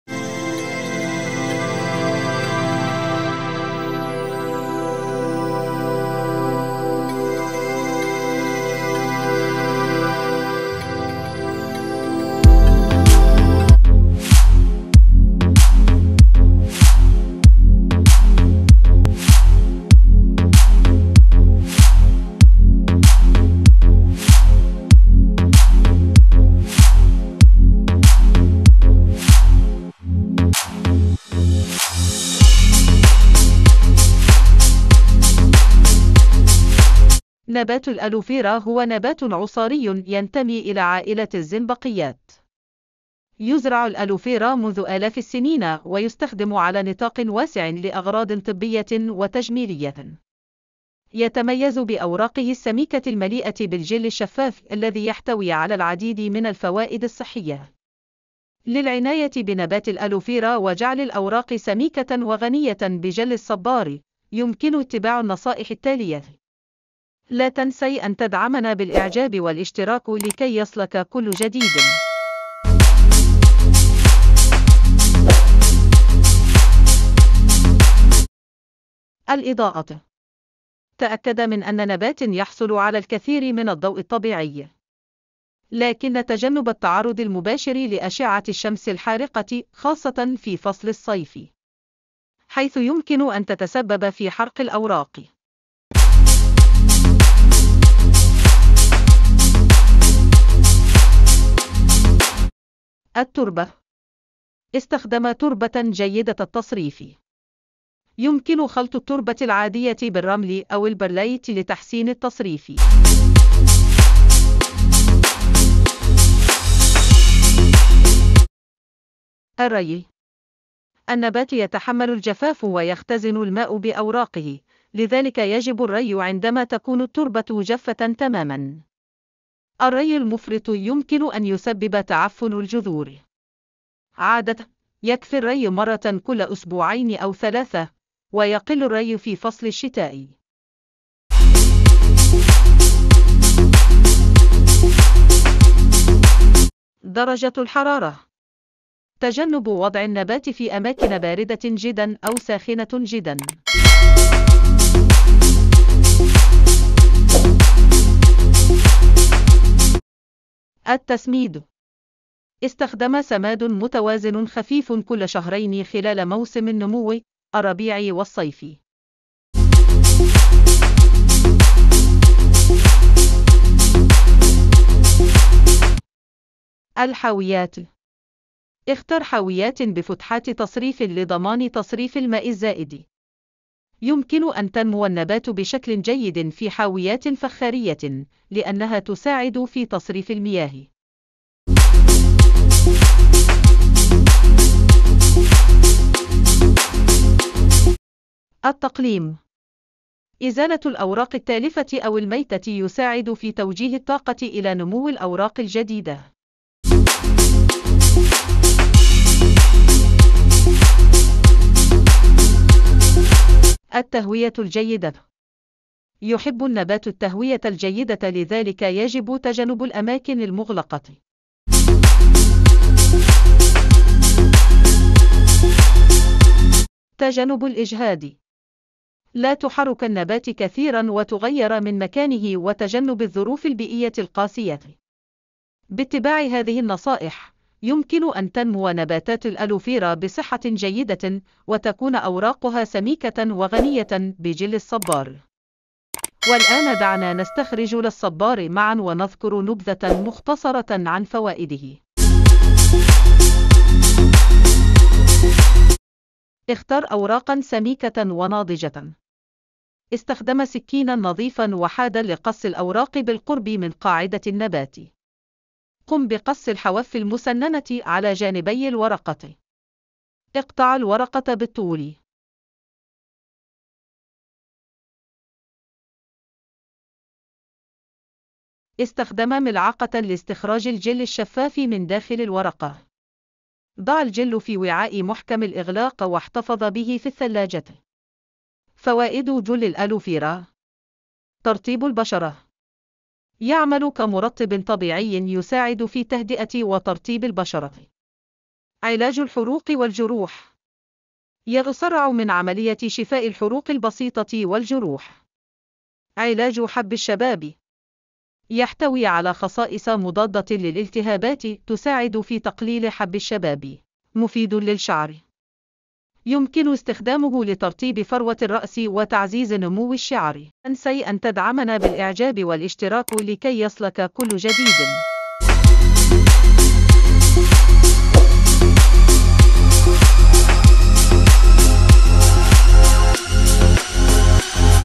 ٩ أسرار لجعل اوراق نبات الألوفيرا سميكة و مليئه بالجل و اسهل طريقة لاستخراج جل الالوفيرا#aloevera
العناية الكاملة بنبات الالوفيرا للحصول علي أوراق مليئة بجل الصبار و اسهل طريقة لاستخراج جل صبار الالوفيرا الذي يستخدم للعناية بالبشرة و الشعر وله استخدامات طبية عديدة و تتضمن طرق العناية ظروف الري و الاضاءة والتربة و التسميد التي يحتاج اليها نبات الألوڤيرا فهو من أجمل العصاريات التي يجب ان تكون موجودة في الحديقة المنزلية